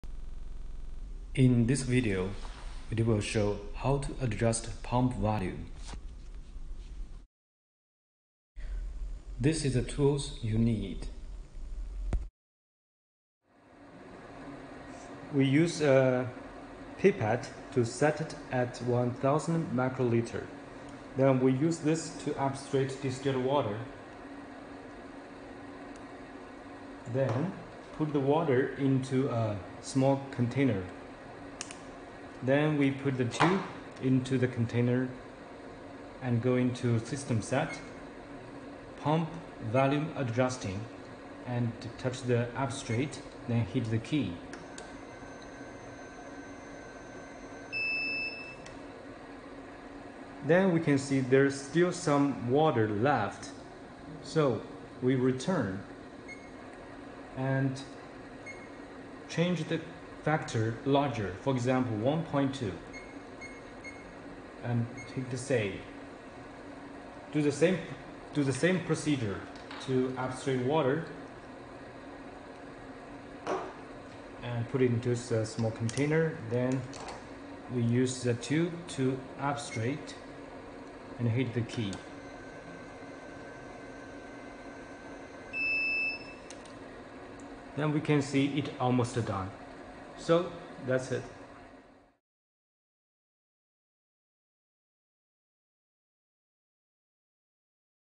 Adjust Pump Volume in RD171
Adjust Pump Volume in RD171. The basic setting for pump volume is 1000 μL. As time passes by, the real volume changes, we need to change the factor to get enough volume.